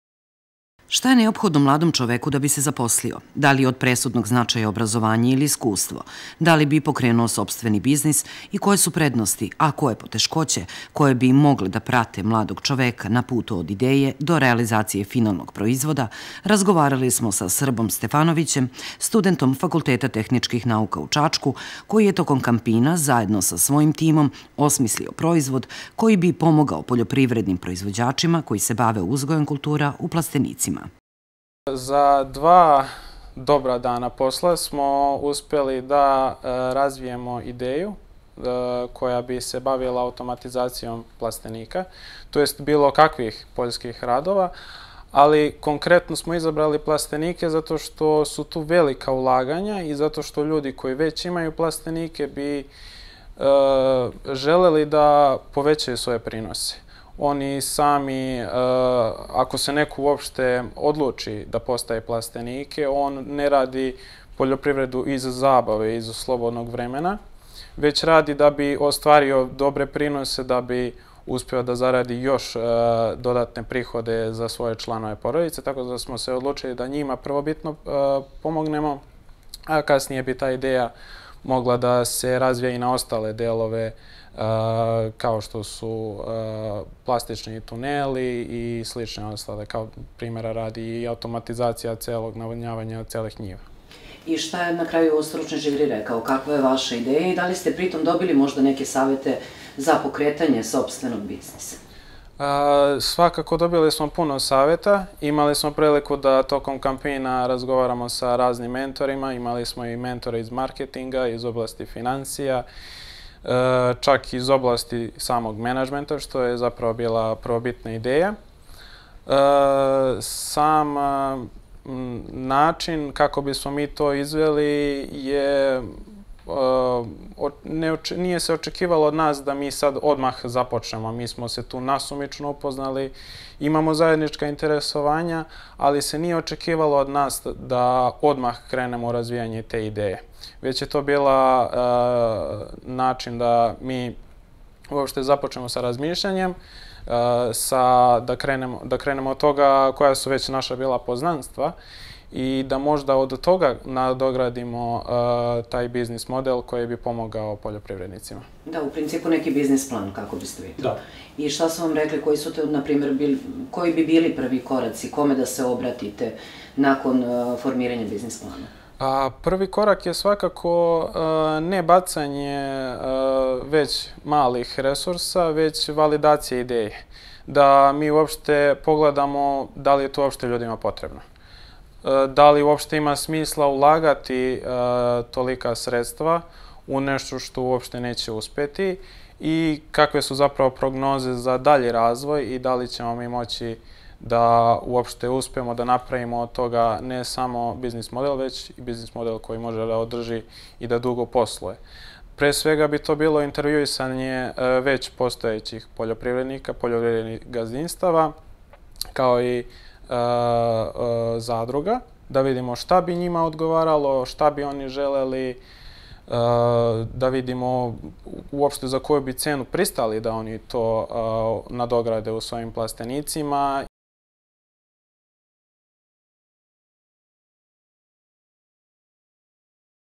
TV Telemark - RAZVOJ PROIZVODA ZA AUTOMATIZACIJU PROCESA U PLASTENICIMA 28 10 2022
Tim koji je nosio naziv “GHT” je grupa mladih koji su učestvovali na Kampinu i tim koji je odneo pobedu prezentovavši ideju o razvoju proizvoda za automatizaciju procesa u plastenicima i drugim poljoprivrednim površinama. Kampino predstavlja samo prvi korak u daljem putu ka razvoju novih tehnoloških rešenja i osnivanju novih startap kompanija u celom regionu.